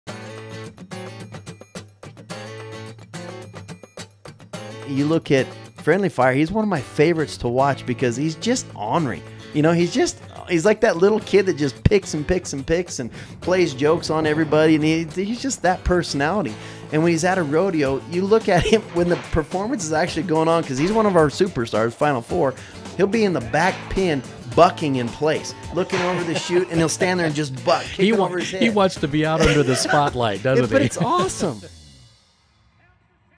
Friendly Fire in Bucking Pens
Friendly Fire a horse with a personality, and he loves to buck.